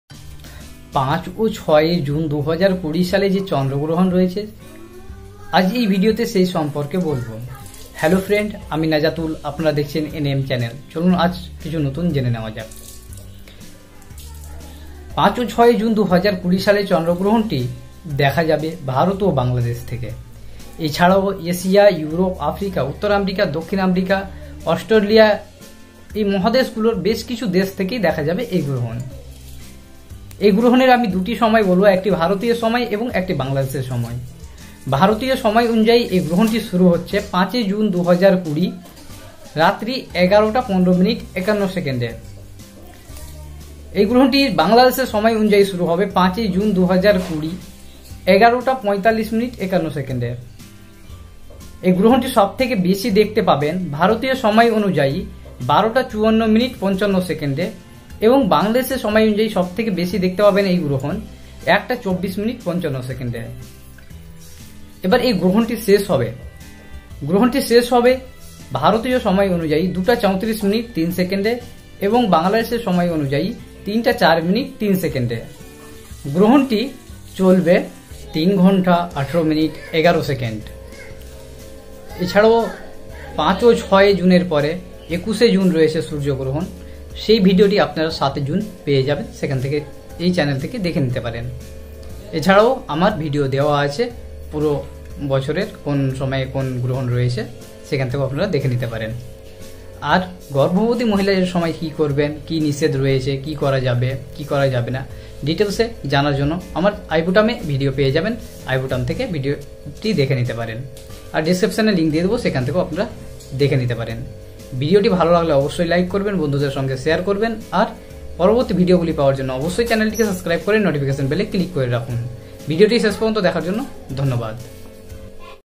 Chandra Grohon 2020 ।। 5 6 Jun Chandra Grohon Time || চন্দ্রগ্রহনের সঠিক সময় ।
5-6 জুন 2020 চন্দ্রগ্রহনের দিন ক্ষণ সময়।
2020 সালের বাকি সব গ্রহণের দিন ক্ষণ ও সময়।

5-6 Jun 2020 Lunar Eclipse
5-6 জুন 2020 চন্দ্রগ্রহণ ভারত ও বাংলাদেশের সময়
5 Jun:-
গ্রহণ শুরু ভারতীয় সময় :- 11:15:51 PM
বাংলাদেশের সময় :- 11:45:51 PM,
6 Jun:-
গ্রহণ সবচেয়ে বেশী হবে ভারতীয় সময়:-12:54:55AM ,
বাংলাদেশের সময়:-01:24:55AM,

গ্রহণ শেষ হবে ভারতীয় সময়:-02:34:03AM বাংলাদেশের সময়:- 03:04:03 AM

বাংলাদেশের যেসব জাগায় দেখা যাবে:-Barisal Division,Chittagong Division,Dhaka Division,
Khulna Division,Mymensingh Division,Rajshahi Division,Rangpur Division,Sylhet Division.

Expand for some cities where penumbral eclipse is visible
Yangon, Myanmar
Lagos, Nigeria
New Delhi, Delhi, India
Bangkok, Thailand
Cairo, Egypt
Sydney, New South Wales, Australia
Jakarta, Jakarta Special Capital Region, Indonesia
Budapest, Hungary
Moscow, Moscow, Russia
Ankara, Turkey
Melbourne, Victoria, Australia
Athens, Greece
Rome, Italy
Tokyo, Japan
Beijing, Beijing Municipality, China
Madrid, Spain
Paris, Île-de-France, France
Brussels, Brussels, Belgium
London, England, United Kingdom